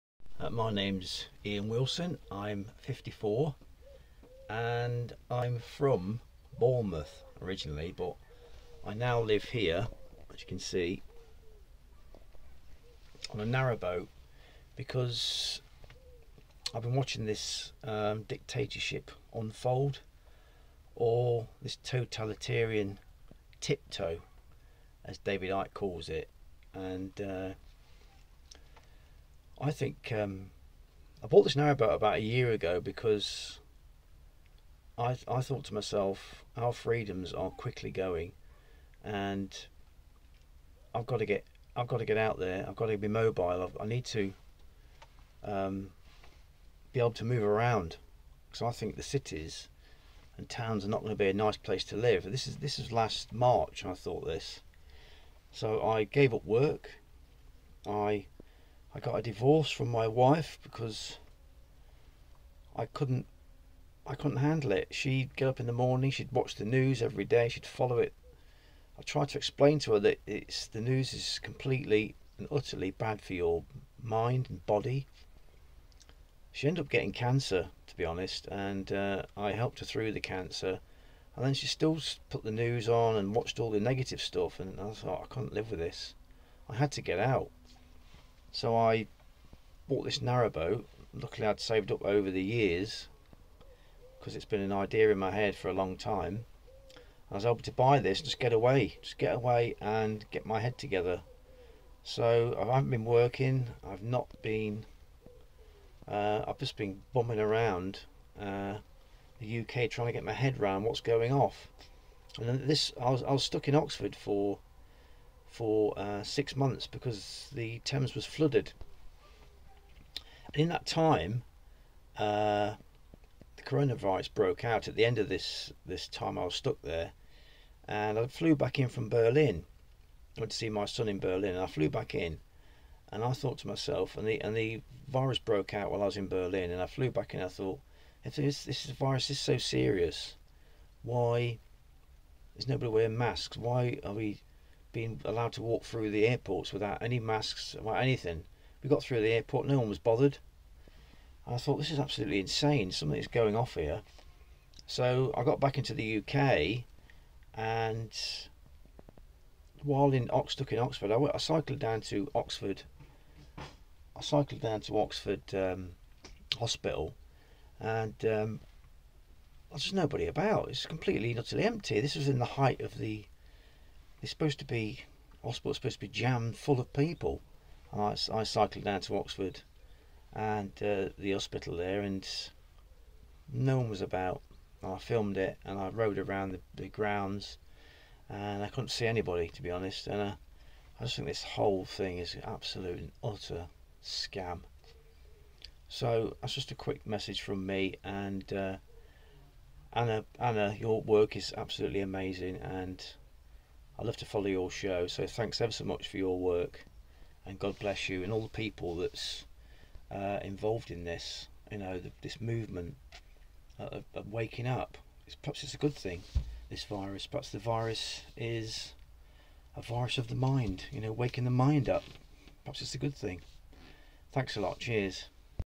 "The Mood of Britain" #35, July 2020 (mirrored for Extinction Rebellion)
Ian Wilson, 54, Bournemouth, Narrowboat Owner  31 July 2020.